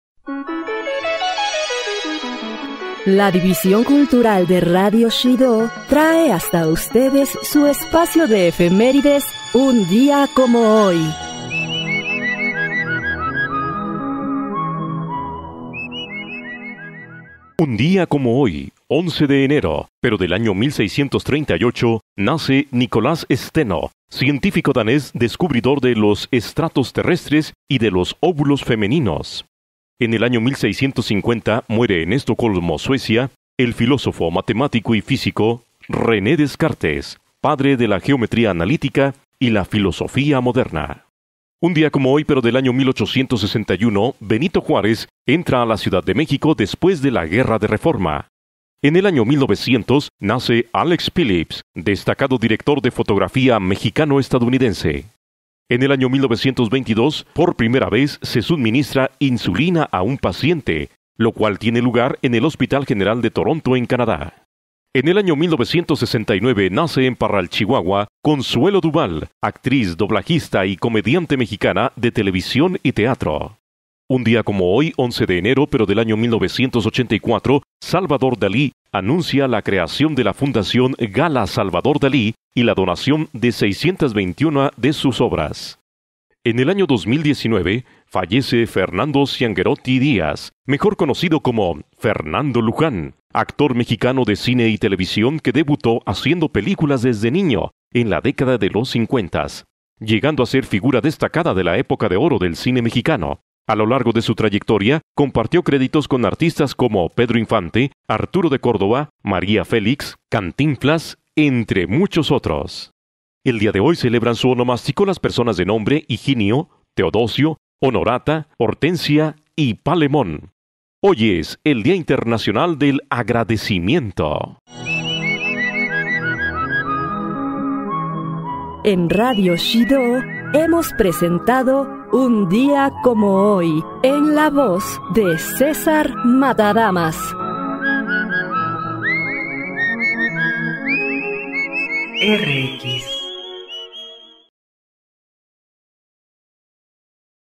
Un Día como Hoy! 11 de Enero
Cápsulas de efemérides en la voz de César Matadamas Hdz. Premio Nacional de Locución 2021.

RENE DESCARTES
De Según Frans Hals - André Hatala [e.a.] (1997) De eeuw van Rembrandt, Bruxelles: Crédit communal de Belgique, ISBN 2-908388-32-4., Dominio público,

ALEX PHILLIPS
Testimonios para la historia del cine mexicano Tomo I, pag. 21. México 1975